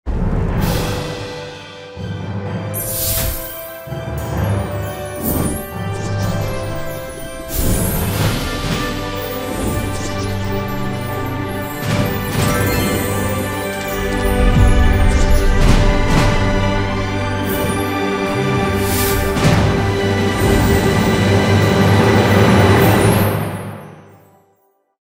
PVR Superplex Ident 2023
Step into a world of wonder and adventure with our latest cinema opener, for PVR Superplex, brought to life in stunning 3D animation, drawing you in and surrounds you with its mesmerizing sights and sounds in Dolby Atmos mix in theatres. Featuring cutting-edge animation and a dynamic sound design, our 3D cinema opener is the perfect way to kick off your movie-going experience at Superplex. So what are you waiting for? Come see the magic of our 3D cinema opener for yourself and discover a whole new world of cinematic wonder!

Agency: Tree Design
VFX Design: Ravi P
3D Animation and compositing: Rahul G
Editor: Praveen C
Sound Engineer - Atmos mix : Aslam K